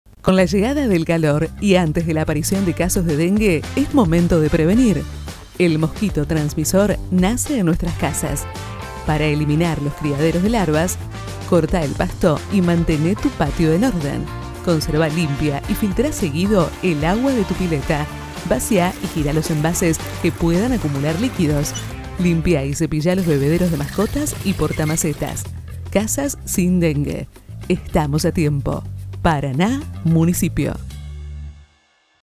Dengue: El momento de prevenir es ahora ⚠️🦟
💬¿Sabías que el mosquito transmisor nace en nuestras casas?

Para evitar su proliferación mantené los espacios verdes en orden: vaciá, girá y limpiá los recipientes que puedan acumular agua.

🏠 Casas sin dengue ❌

Más info ℹ️
parana.gob.ar